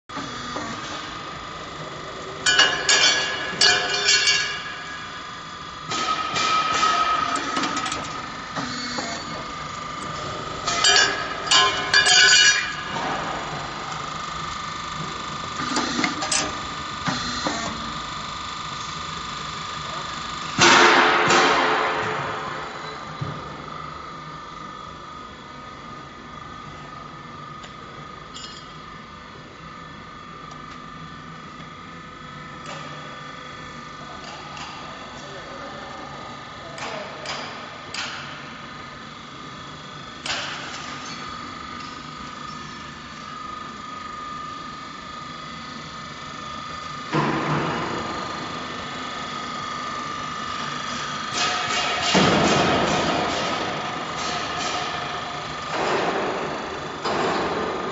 Roller shutter door roll forming machine/rolling door making machine/ door rolling machine/yingyee
here is the machine video for the roller shutter door, it is one smalm machine, speed could be 10m/min. It is easy to control.